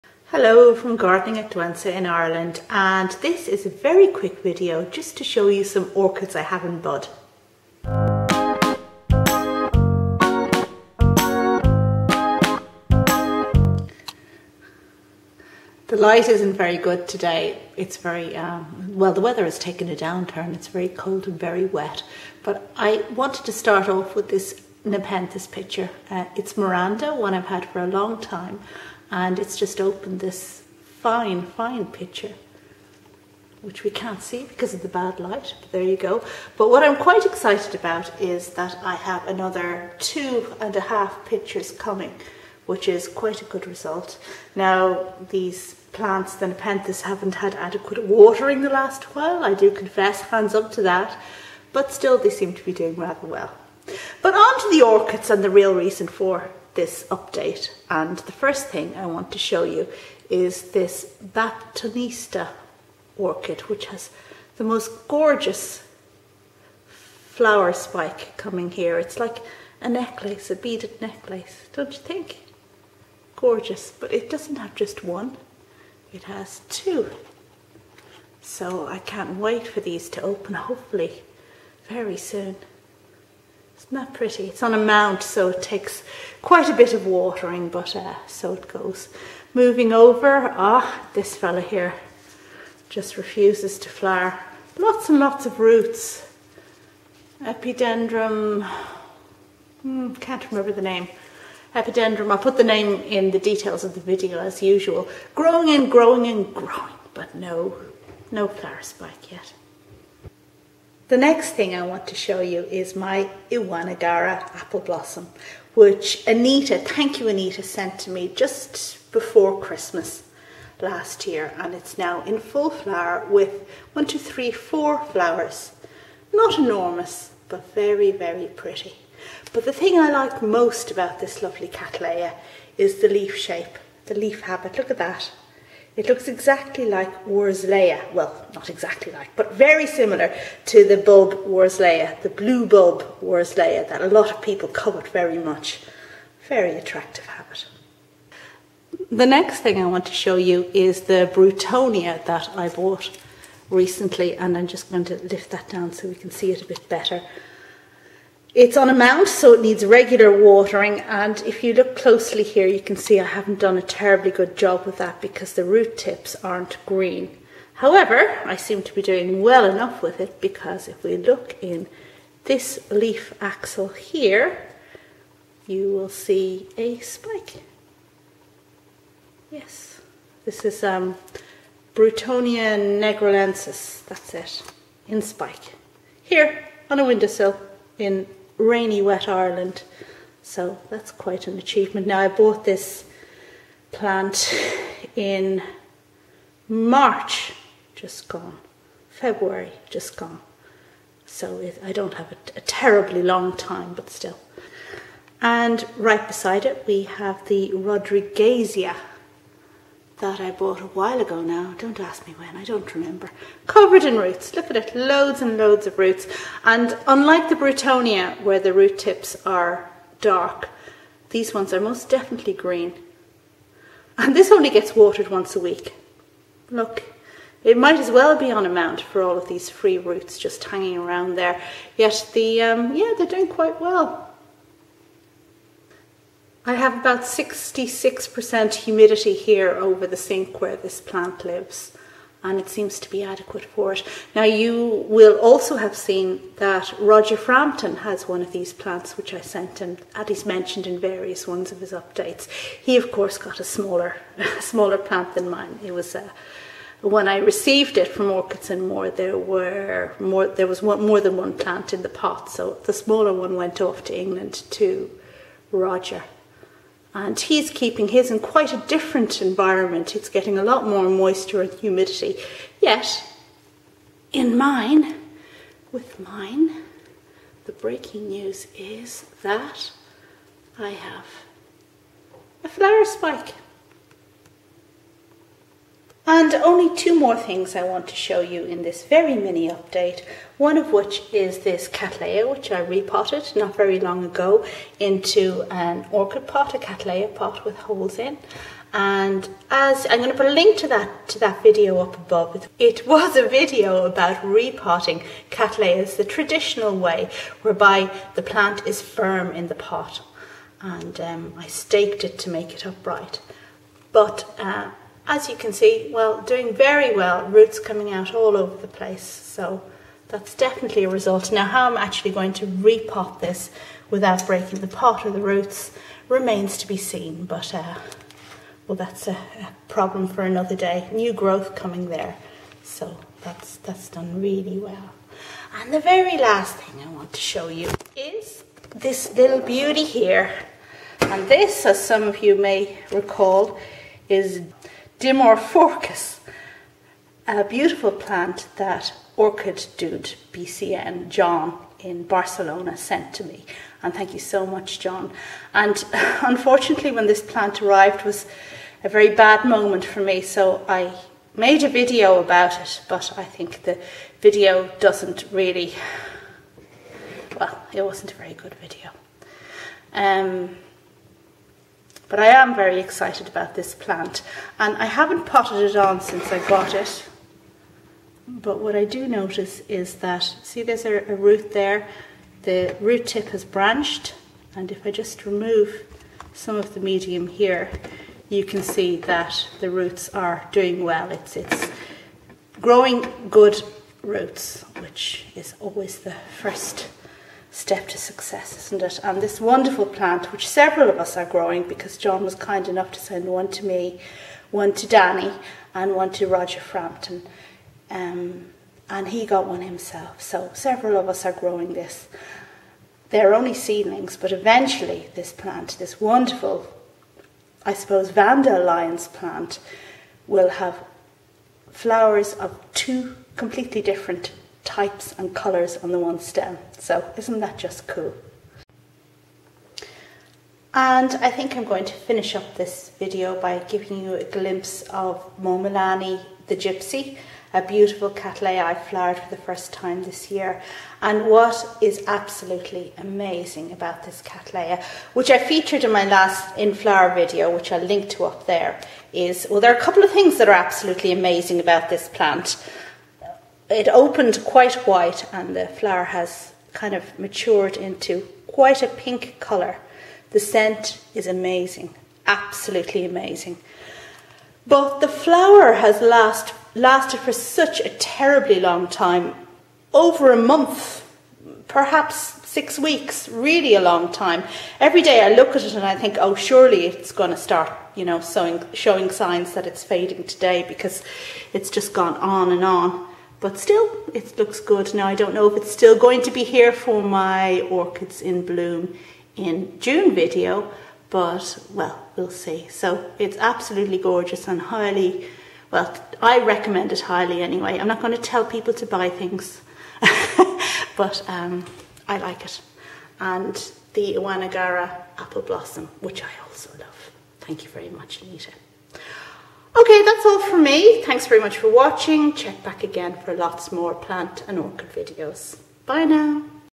Kitchen Windowsill Orchid Update - root tips, humidity & buds
This video shows my kitchen window orchids in spike/bud.

Plants in order of video appearance are...
Nepenthes 'Miranda'
Baptistonia truncata
Epidendrum parkinsonianum
Iwanagaara /Jackfowlieara Appleblossom
Broughtonia negrilensis
Rodriguezia leeana
Rhyncholaeliocattleya George King 'Serendipity'
Dimorphorchis iowii
Brassiolaeliocattleya Momilani Rainbow 'The Gypsy'

For fun, why not try my orchid quiz...

If you liked this video, you'll love my book, Journal of an Irish Garden.